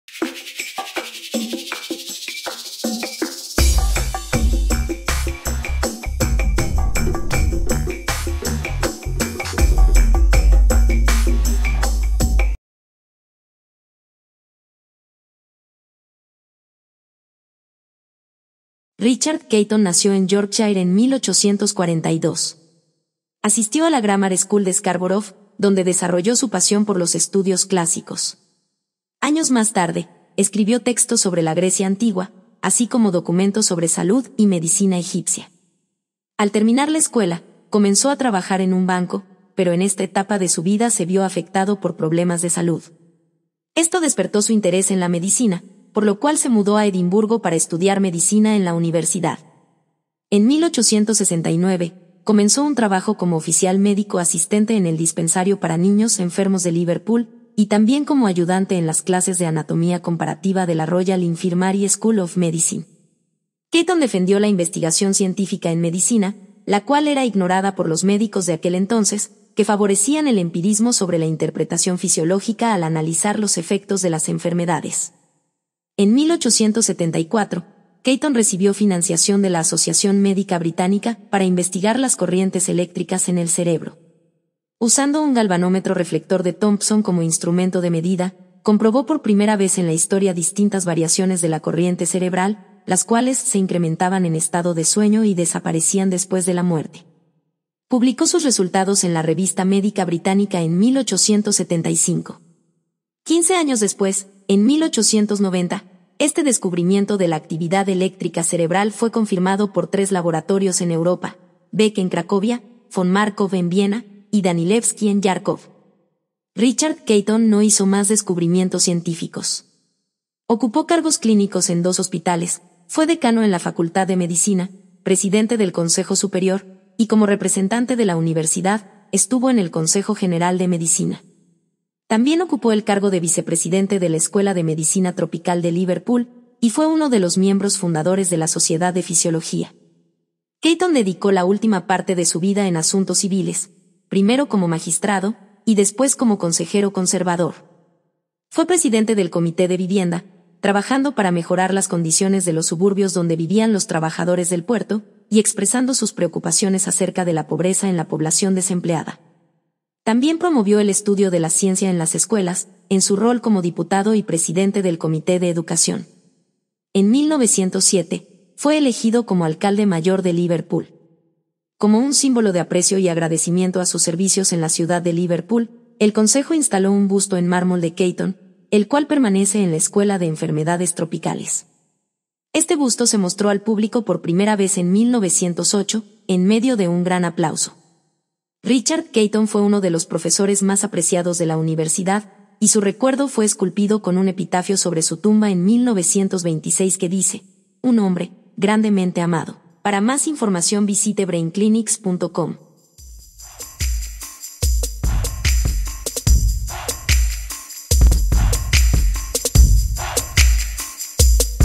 Pioneros de la electroencefalografía -EEG, Episodio 2 Richard Caton
Presentamos este video como parte de nuestra serie “Pioneros de la electroencefalografía -EEG”. Richard Caton es reconocido por haber desarrollado el primer EEG (proyectando una señal de EEG sobre una pared usando un galvanómetro de espejo).

Se autoriza el uso gratuito de estos videos para propósitos científicos y educativos (para uso comercial, por favor contáctenos).

Este episodio ha sido revisado por el profesor A.M.L. Coenen (Radboud University).
Traducido por Alexander Cerquera, Ph.D.

Conceptualizado y producido por Brainclinics Insights.